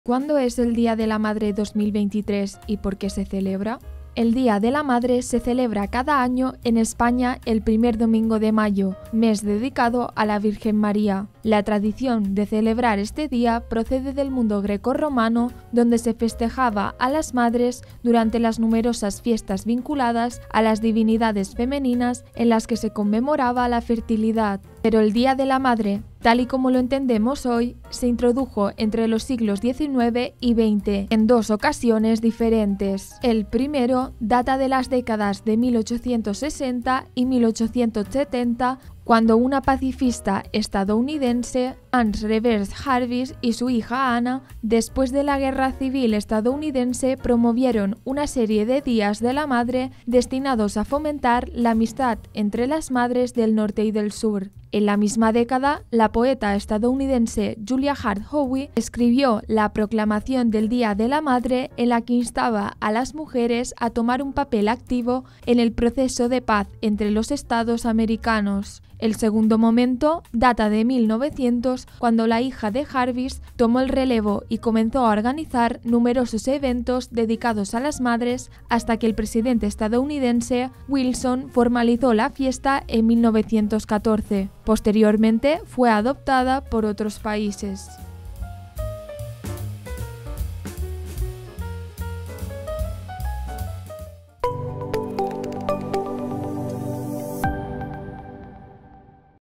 ¿Cuándo es el Día de la Madre 2023 y por qué se celebra?
El día de la madre se celebra cada año en España el primer domingo de mayo, mes dedicado a la virgen María. La tradición de celebrar este día procede del mundo greco-romano donde se festejaba a las madres durante las numerosas fiestas vinculadas a las divinidades femeninas en las que se celebraba la fertilidad. Pero el día de la madre tal y como lo entendemos hoy, se introdujo entre los siglos XIX y XX , en dos ocasiones diferentes.El primero data de las décadas de 1860 y 1870 cuando una pacifista estadounidense, Ann Reeves Jarvis y su hija Anna. Después de la Guerra Civil Estadounidense, promovieron una serie de Días de la Madre destinados a fomentar la amistad entre las madres del Norte y del Sur.  En la misma década, la poeta estadounidense Julia Ward Howe escribió la «Proclamación del Día de la Madre», en la que instaba a las mujeres a tomar un papel activo en el proceso de paz entre los estados americanos. El segundo momento, data de 1900 cuando la hija de Jarvis tomó el relevo y comenzó a organizar numerosos eventos dedicados a las madres, hasta que el presidente estadounidense Woodrow Wilson formalizó la fiesta en 1914. Posteriormente, fue adoptada por otros países.